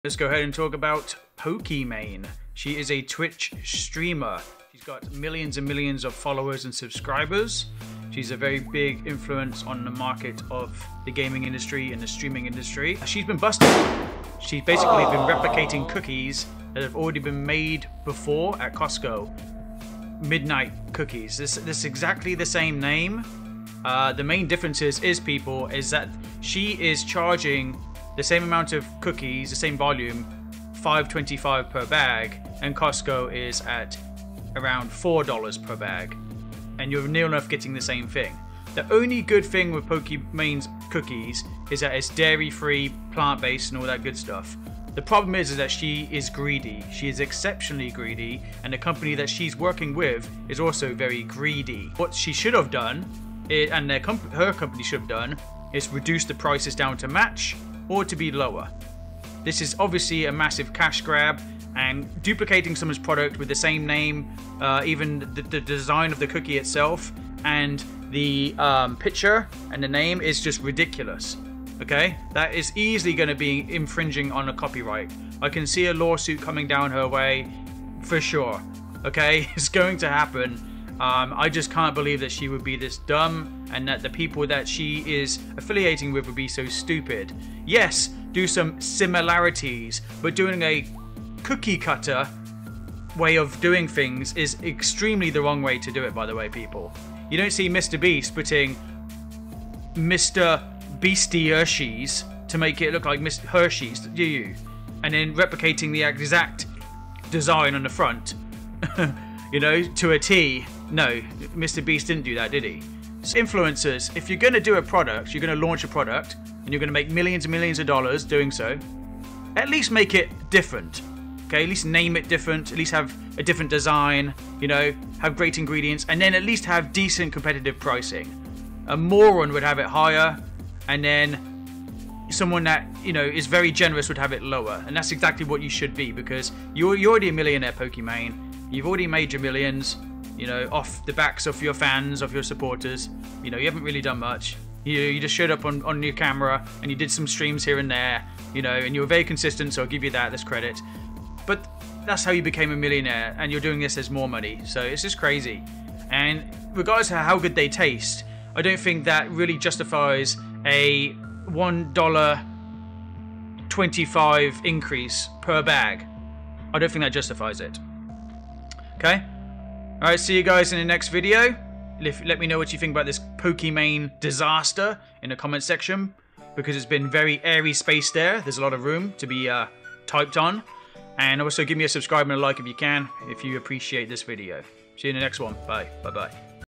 Pokimane Scams Millions Of Dollar`s Over Bloody Cookies!
I will present facts, however i am  NOT going to swear, even though this rages me, with the amount of blatant rip off you tubers and greedy people in this world.

Member up be an official Z GAMER Today!